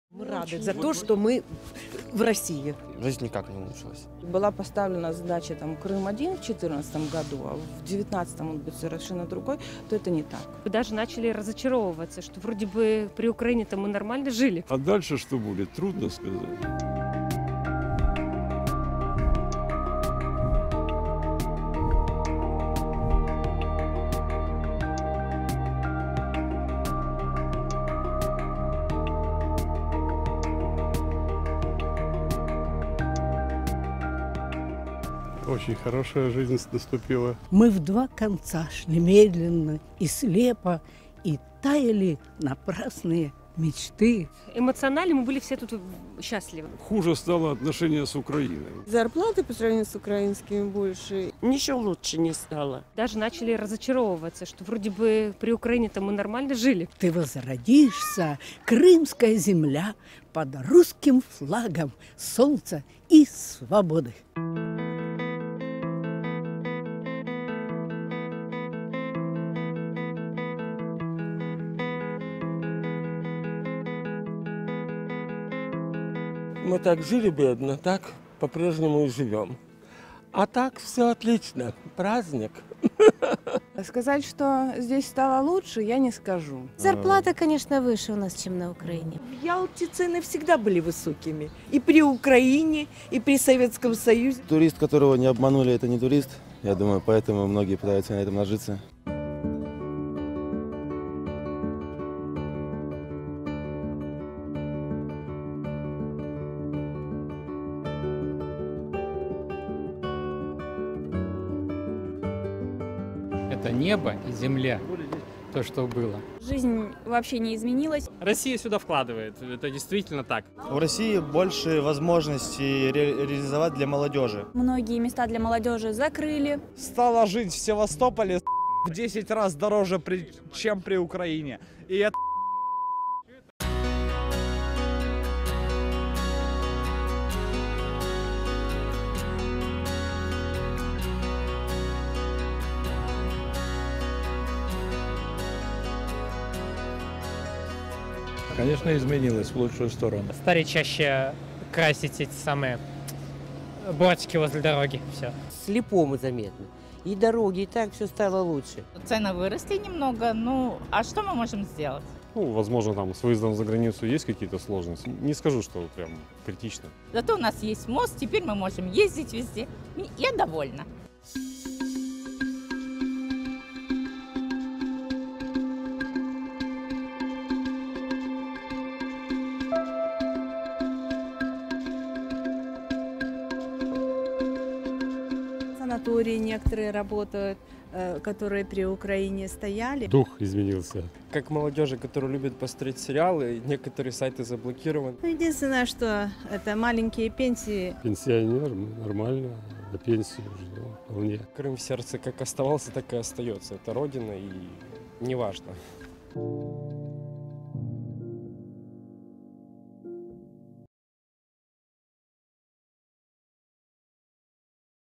Қримда Россия НАТОни ғафлатда қолдирди, бугун қримликлар нима дейди? - BBC Uzbek
Би-би-си билан суҳбатда НАТОнинг собиқ бош котиби Андерс фог Расмуссен 2014 йили Қримни босиб олиш билан Россия Шимолий Иттифоқни ғафлатда қолдирганини тан олди.

Қрим ярим ороли Россияга аннексия қилингани 5 йиллиги муносабати билан Би-би-си Рус хизмати қримликларнинг фикрларини ўрганди.